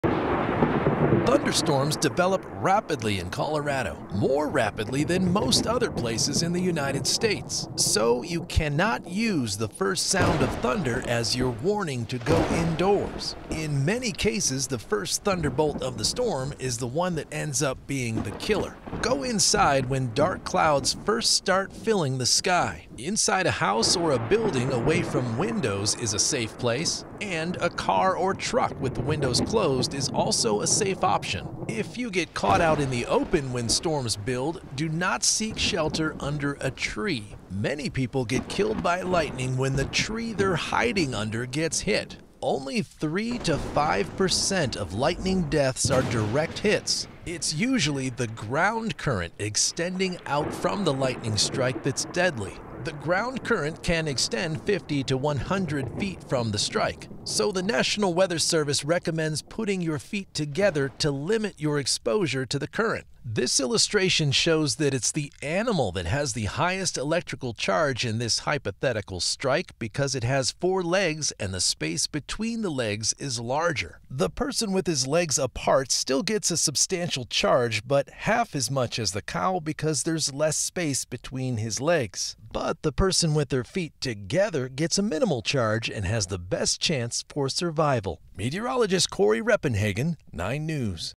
Here's how to stay safe during lightning season in Colorado
A rancher in Jackson County, a loving husband and father, was killed by a lightning strike on May 26.

9NEWS (KUSA) is located in Denver, Colorado.